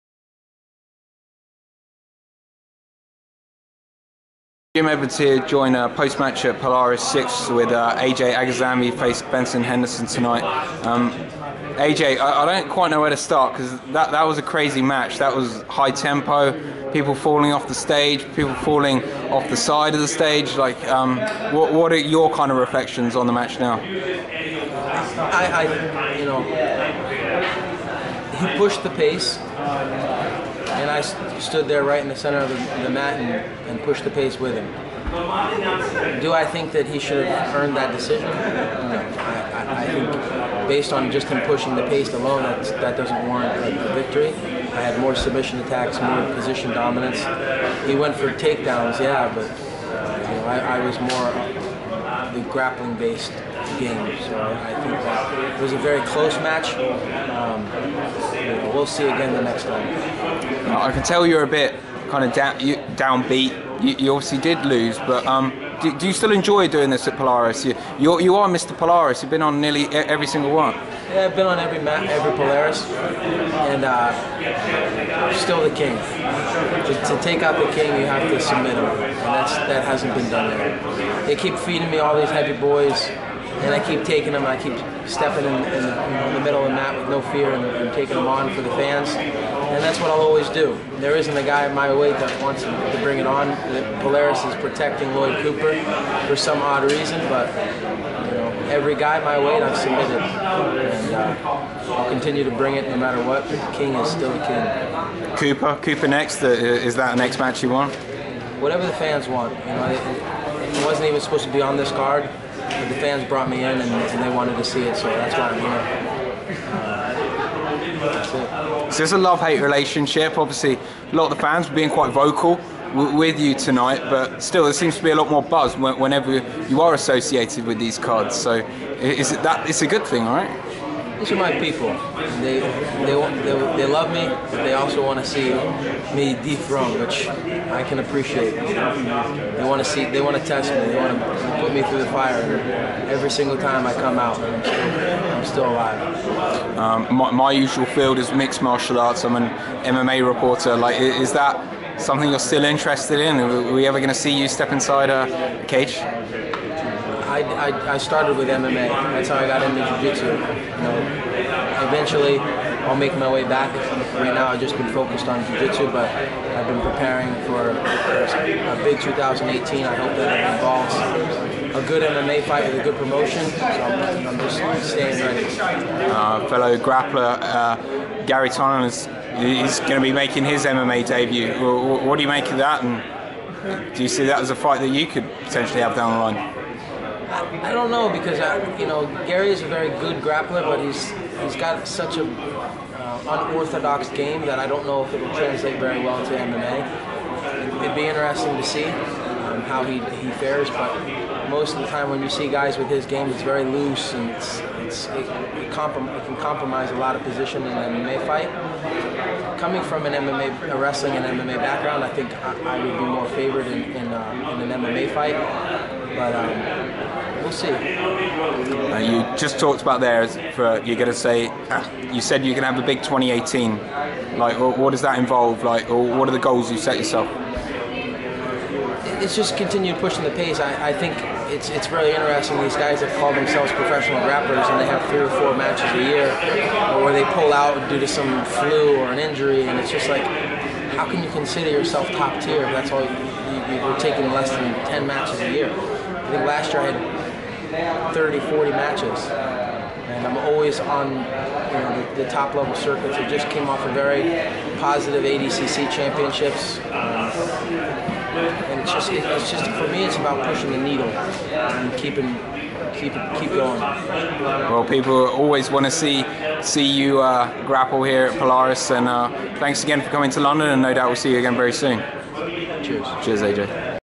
AJ Agazarm Expects To Have An MMA Fight In 2018 l
AJ Agazarm spoke to MMANytt.com following his loss to Benson Henderson at Polaris 6 last evening. He spoke about the match and when he expects to be making his MMA debut.